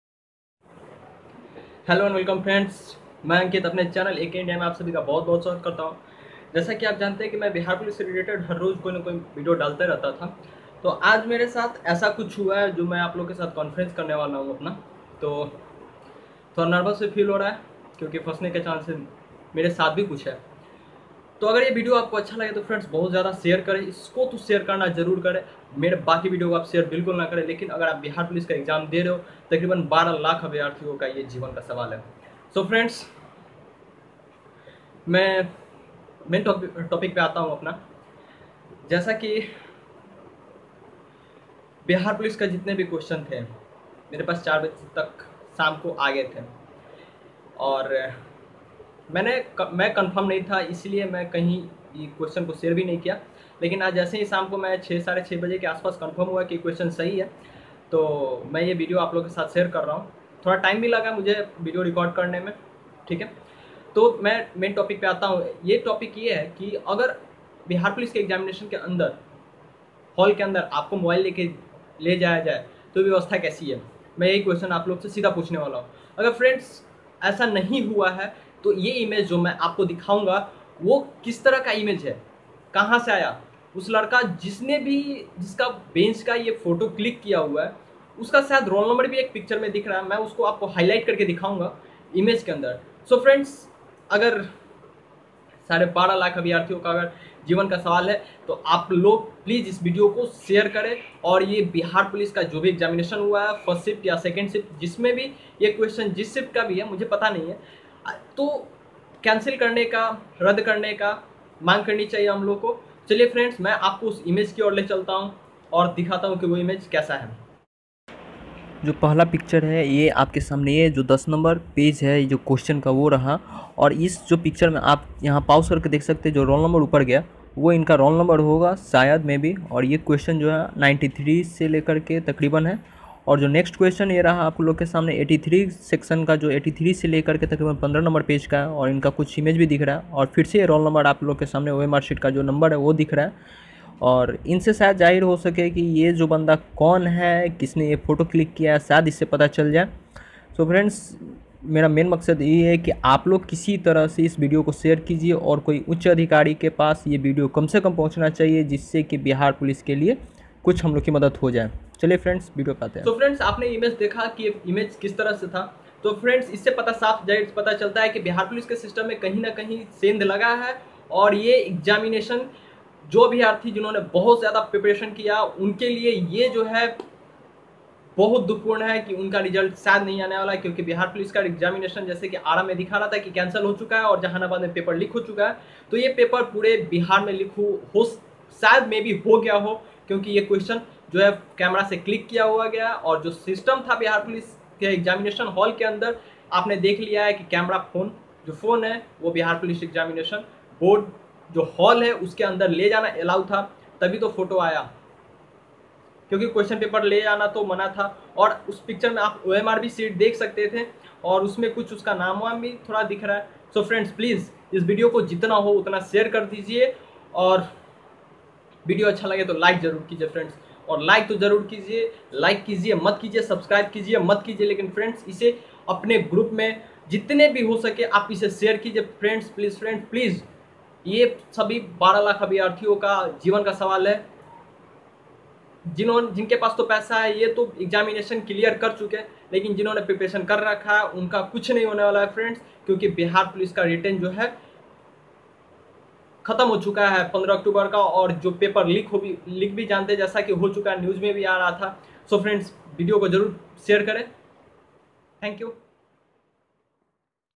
Bihar police constable written exam viral video
Bihar police constable exam 2017 viral video.
Paper leak huaa dosto plz isse itna share kare ki koi adhikari tk ye video pahuch jaye.